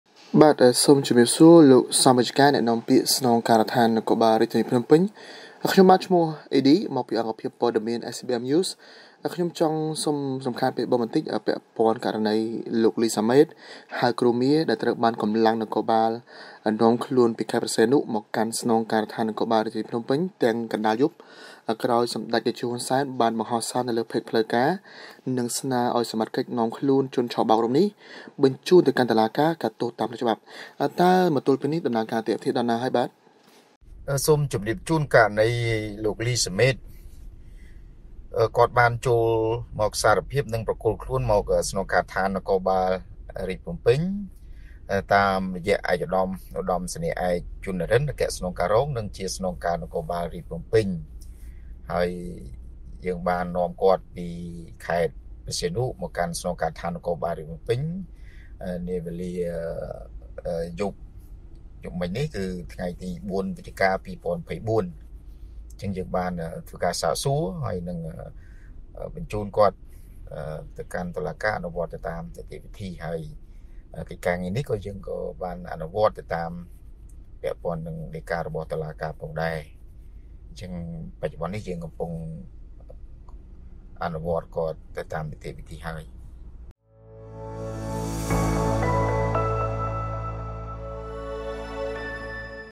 មន្ត្រីនាំពាក្យ អះអាងថា សមត្ថកិច្ចកំពុងសាកសួរព័ត៌មាន និងបន្តនីតិវិធីលើលោក លី សាម៉េត ត្រៀមបញ្ចូនទៅ
មន្ត្រីនាំពាក្យ អះអាងថា សមត្ថកិច្ចកំពុងសាកសួរព័ត៌មាន និងបន្តនីតិវិធីលើលោក លី សាម៉េត ត្រៀមបញ្ចូនទៅតុលាការភ្នំពេញ ក្រោយនាំខ្លួនពីខេត្តព្រះសីហនុSBMNEWS
ផលិតដោយសារព័ត៌មានអេឡិចត្រូនិកស្កាយប្រាឌឺមេឌា